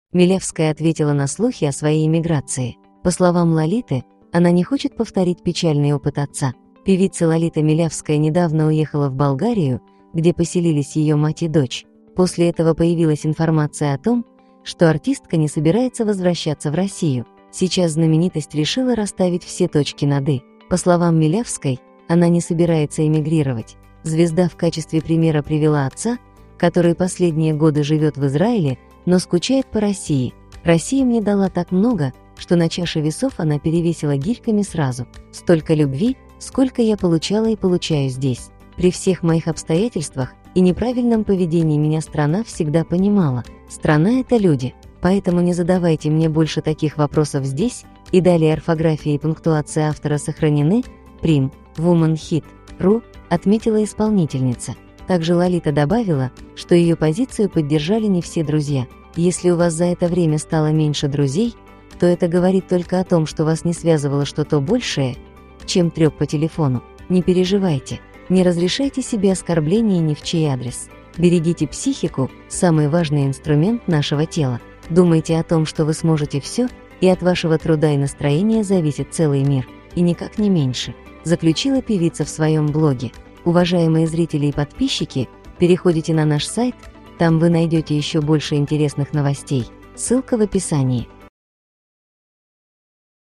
Милявская ответила на слухи о своей эмиграции
По словам Лолиты, она не хочет повторить печальный опыт отца.

Певица Лолита Милявская недавно уехала в Болгарию, где поселились ее мать и дочь. После этого появилась информация о том, что артистка не собирается возвращаться в Россию.

Сейчас знаменитость решила расставить все точки над «i». По словам Милявской, она не собирается эмигрировать. Звезда в качестве примера привела отца, который последние годы живет в Израиле, но скучает по России.